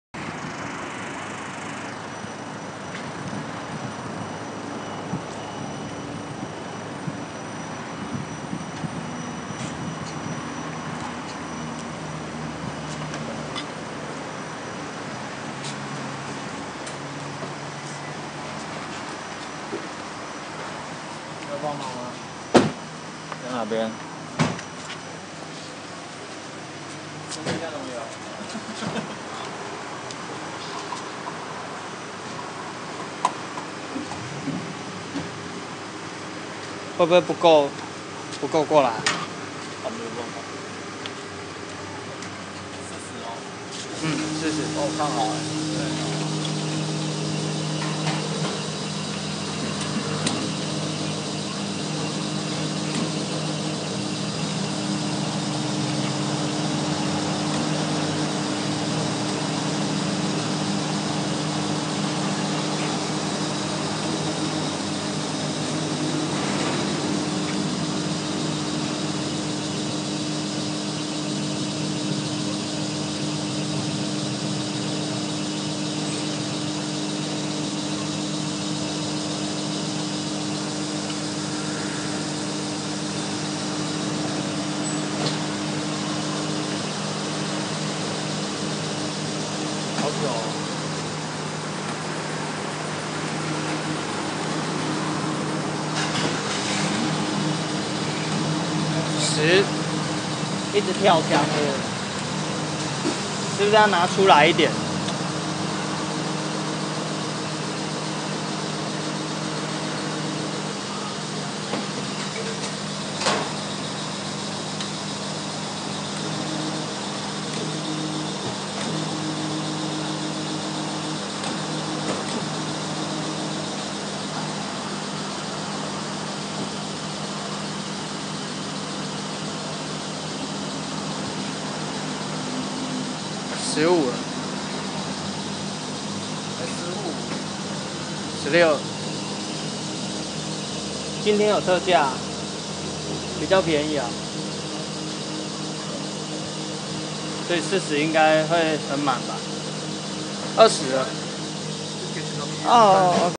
Australia澳洲背包客 - 澳洲加油站
澳洲加油站一律需要自己來，加完還要自己進去商店內付錢！